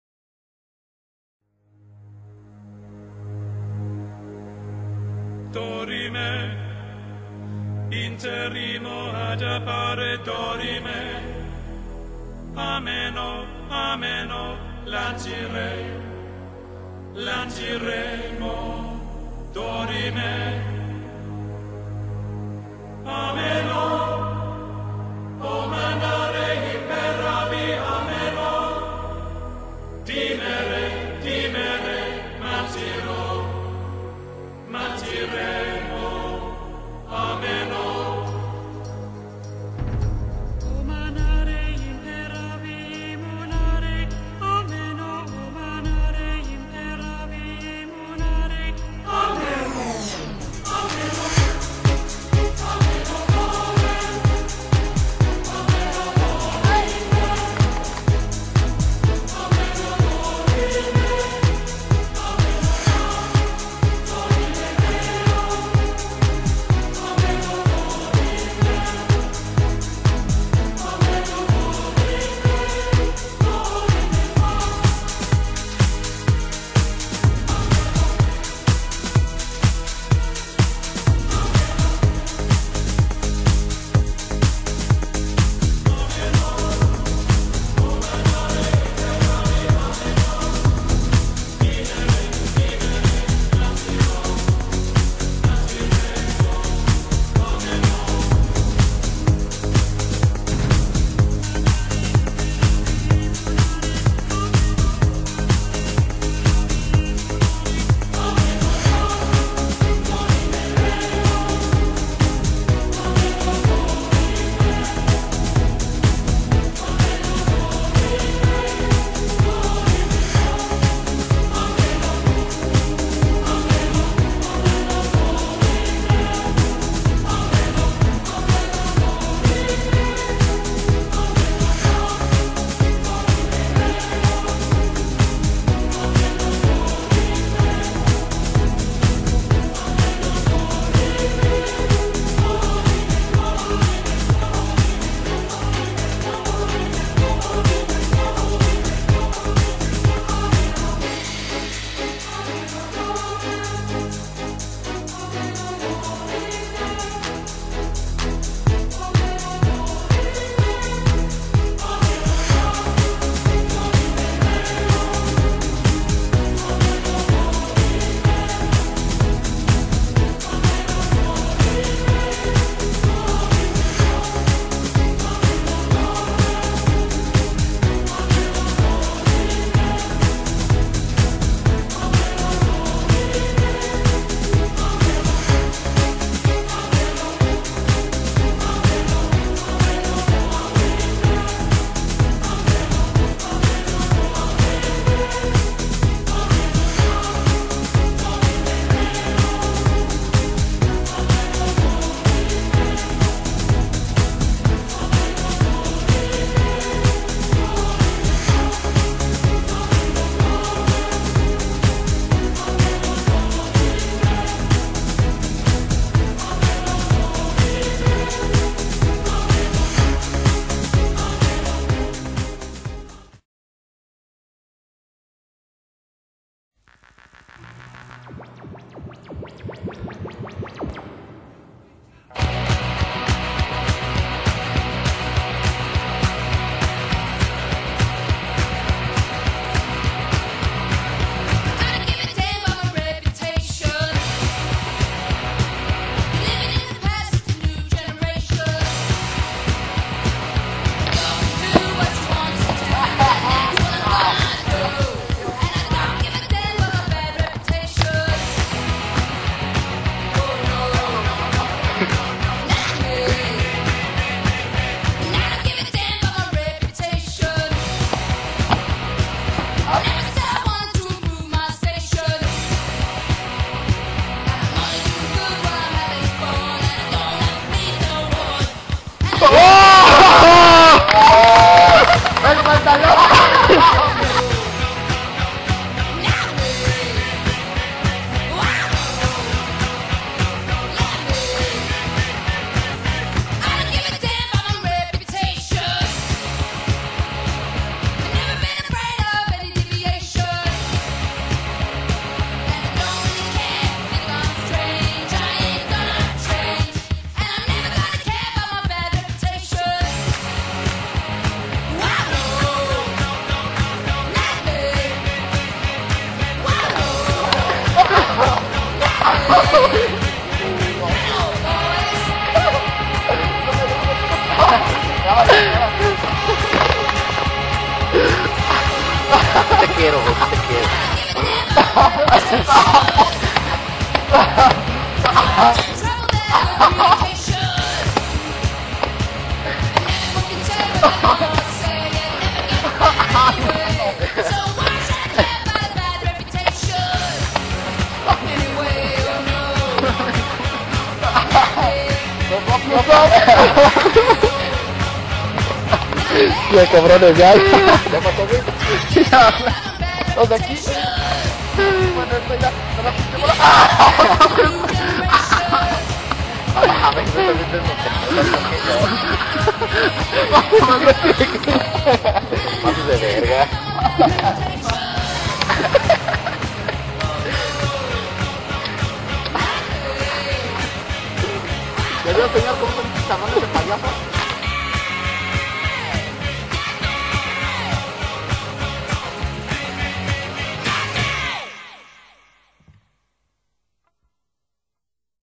Free Running SP
Este es un video de una recopilacion del entrenamiento sp hubiera quedado mucho mejor pero por las putas carreras de estos weyes asi quedo chekenlo